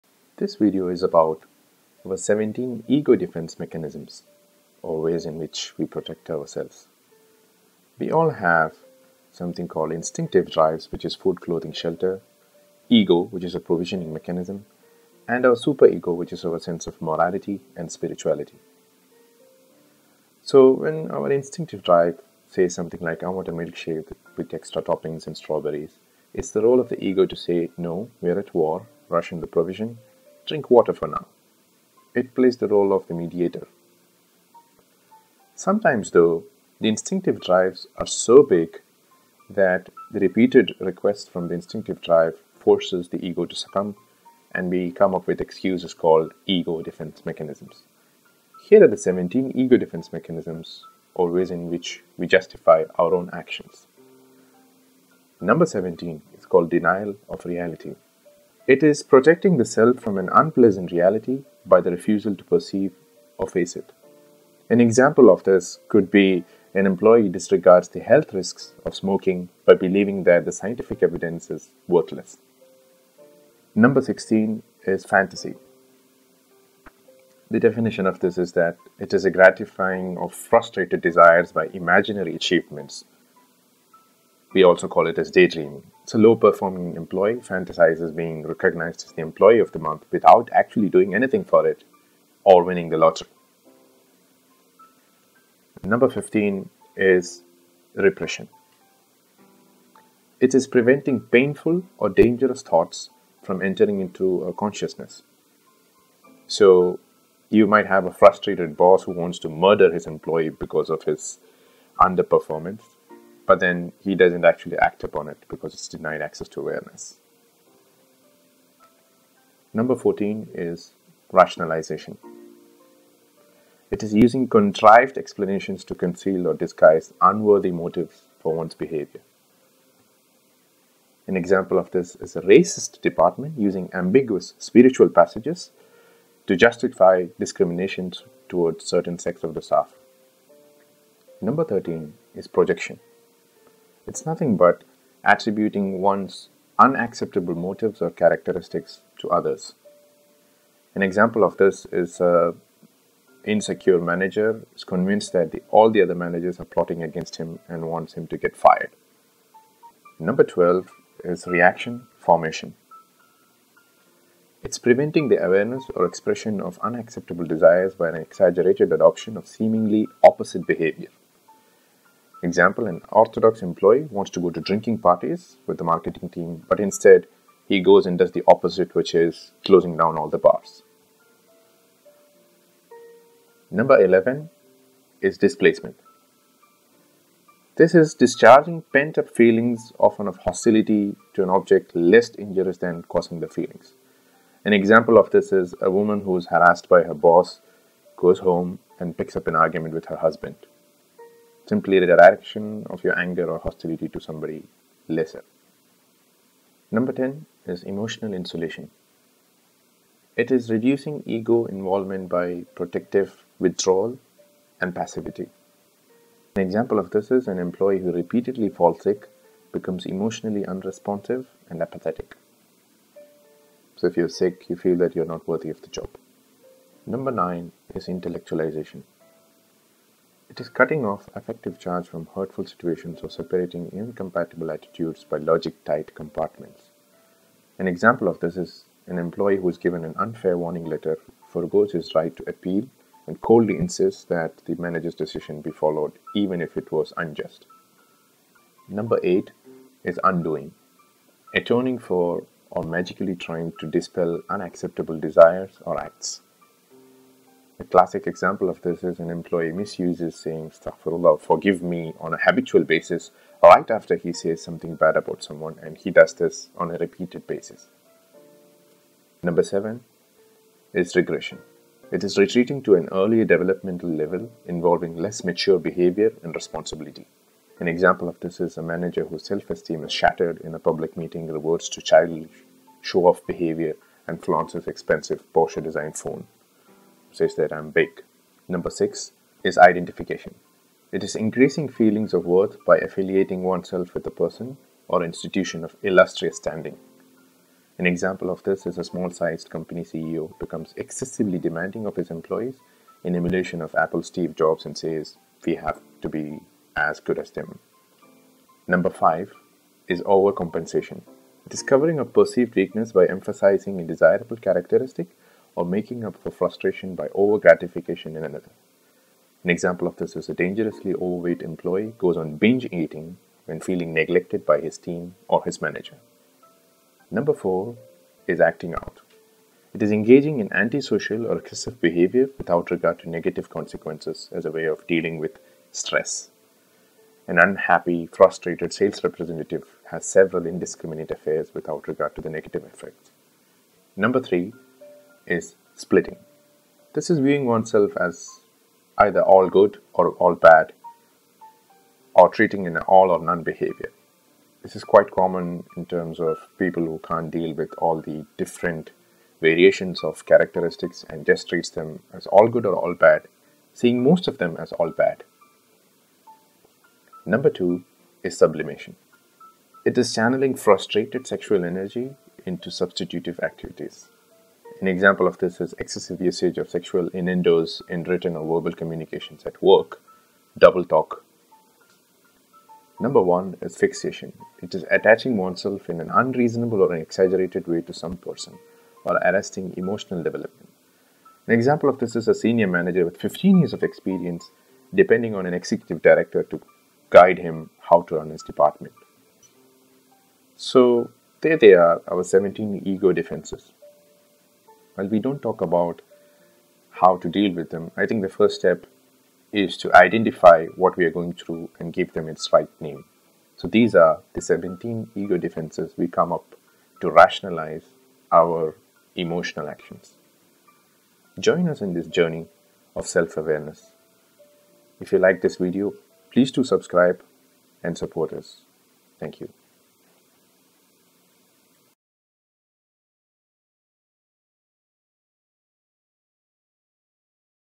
17 Ego Defense Mechanisms used at Work
17 Ego Defense Mechanisms used at Work - How we justify our actions

Here is a look at ways in which our Ego justifies the actions of our Instinctive Drive and actually succumbs to the needs of it.

DENIAL OF REALITY Protecting the self from an unpleasant reality by the refusal to perceive or face it. An employee disregards the health risks of smoking cigarette, by believing that the scientific evidence is worthless.

FANTASY Gratifying frustrated desires by imaginary achievements A low performing employee fantasizes being recognized as the Employee of the Month and winning the lottery.

REPRESSION Preventing Painful or dangerous thoughts from entering consciousness A frustrated boss’s murderous impulses towards his under-performing employee are denied access to awareness

RATIONALIZATION Using contrived “explanations” to conceal or disguise unworthy motives for one’s behavior A racist HR department uses ambiguous passages from scripture to justify discrimination towards certain sects of the staff.

PROJECTION Attributing one’s unacceptable motives or characteristics to others An insecure manager is convinced that all the other managers are plotting against him to get him fired.

REACTION FORMATION Preventing the awareness or expression of unacceptable desires by an exaggerated adoption of seemingly opposite behavior An orthodox employee wants to go to drinking parties with the marketing team, instead he goes on to file a petition to close down all the bars.

DISPLACEMENT Discharging pent-up feelings, often of hostility, on objects less dangerous than those arousing the feelings A woman harassed by her boss at work initiates an argument with her husband.

EMOTIONAL INSULATION Reducing ego involvement by protective withdrawal and passivity. An employee who repeatedly falls sick and undergoes hospitalization, becomes emotionally unresponsive and apathetic

INTELLECTUALIZATION (ISOLATION) Cutting off affective charge from hurtful situations or separating incompatible attitudes by logic-tight compartments. An employee given an unfair warning letter by his manager, forgoes his right to appeal and coldly insists that the manager’s decision be followed blindly.

UNDOING Atoning for or magically trying to dispel unacceptable desires or acts An employee misuses saying “astagfirullah” (Forgive me lord) on a habitual basis right after he says something bad about someone.

REGRESSION Retreating to an earlier developmental level involving less mature behavior and responsibility A manager who is cornered an ridiculed in a high level management meeting, throws all paper evidences on the face on his staff and shows off his Porsche design Phone

IDENTIFICATION Increasing feelings of worth by affiliating oneself with a person or institution of illustrious standing A small sized company CEO becomes excessively demanding of his employees in emulation of Apple’s Steve jobs

OVERCOMPENSATION Covering up perceived weaknesses by emphasizing a desirable characteristic or making up for frustration by over gratification in another A dangerously over-weight employee goes on binge eating when feeling neglected by the team or manager.

ACTING-OUT Engaging in antisocial or excessive behavior without regard to negative consequences as a way of dealing with emotional stress An unhappy, frustrated sales representative has several indiscriminate affairs without regard to the negative effects of the behavior

SPLITTING Viewing oneself or others as all good or bad without integrating positive or negative qualities into the evaluations; reacting to others in an “all or none”, manner rather than considering the full range of their qualities. A conflicted manager does not recognize individual qualities or characteristics of his employees. Instead, he views them as all good or all bad, seeing most of them as all bad.

SUBLIMATION Channeling frustrated sexual energy into substitutive activities Excessive usage of sexual innuendos in written or verbal communication. Double-talk.

FIXATION Attaching oneself in an unreasonable or exaggerated way to some person, or arresting emotional development on a childhood or adolescent level A senior manager with 10 years’ experience depending on director on guidance on how to run his department.